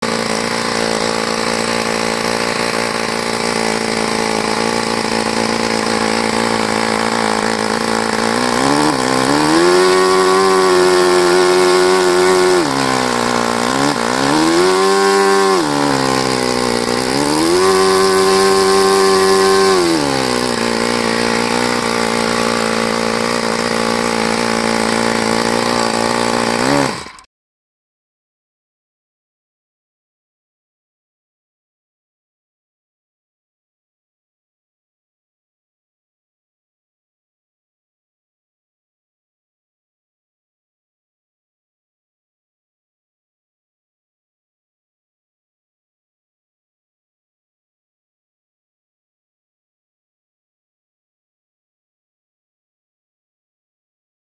SAITO FA30 RUNNING LIKE A SWISS CLOCK!
This little beauty popped right off and never looked back.